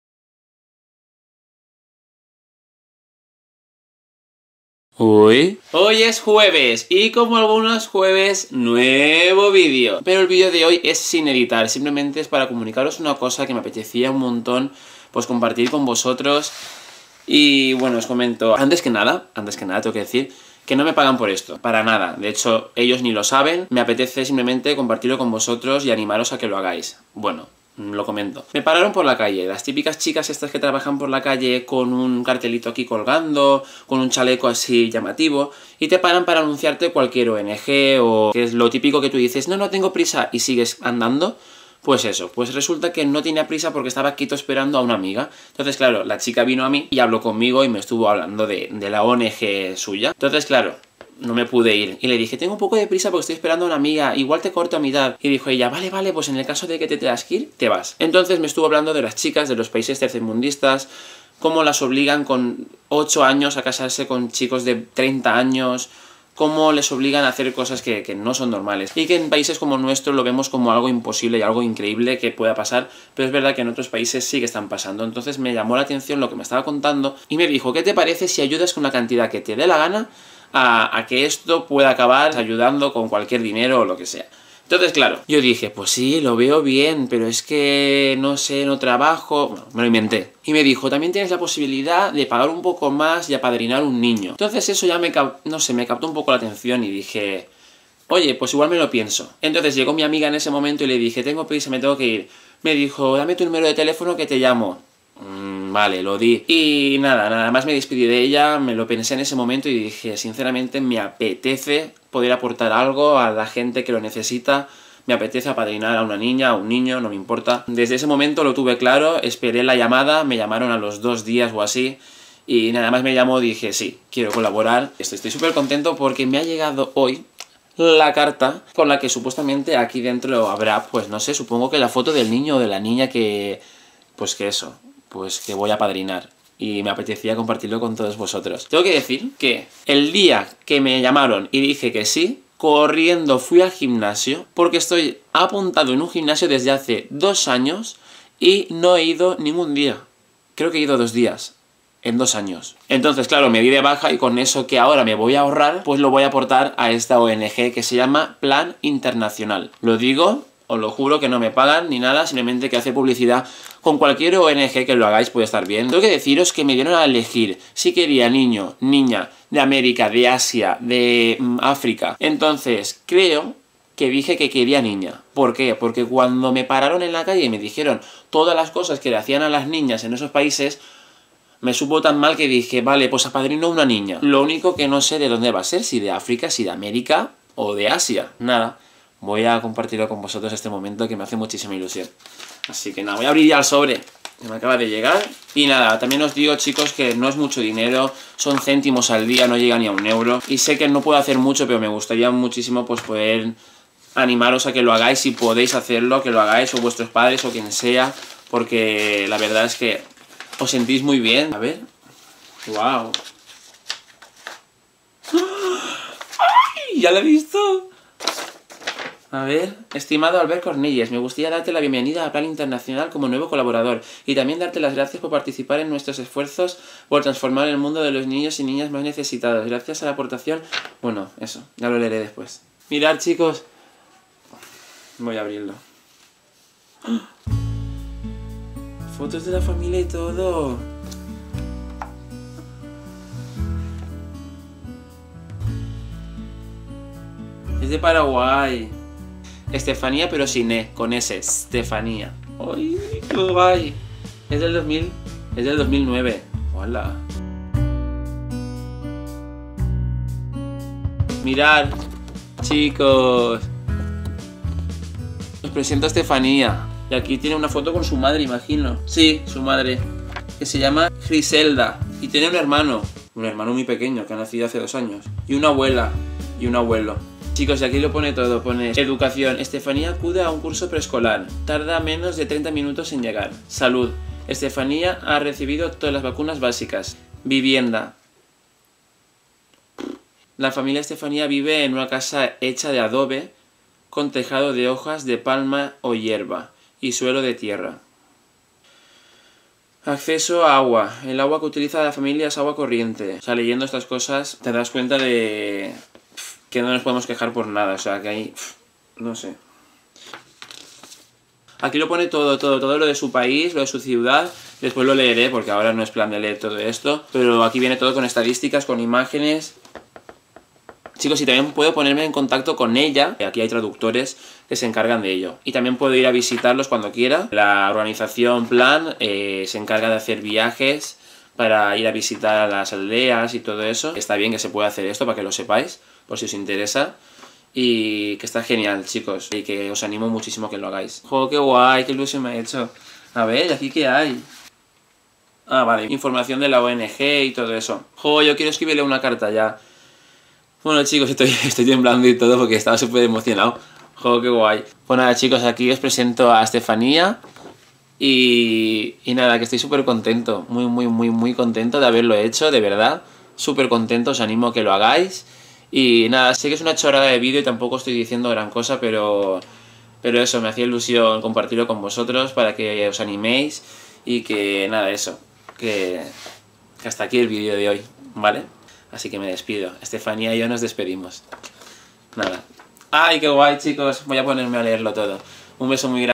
OS PRESENTO A STEFANIA (APADRINA UN NIÑO) | Uy Albert!
Este video no es una colaboración con ninguna página!
Hago publicidad de "PLAN INTERNACIONAL" porque es la ONG con la que he apadrinado a una niña (Stefania), pero esto lo hago para ayudar, y para animaros para que también lo hagáis! No me importa si lo hacéis con "Plan Internacional" o con cualquier otra ONG como "Vicente Ferrer"! con tal de que, si podéis, lo hagáis! porque es por una buena causa!
GRACIAS!

UY!

También podéis seguirme por:

• Vine: UY.Albert
• UpClose: UyAlbert

UY ALBERT!
Apartado de Correos 17015
28080 Madrid (España)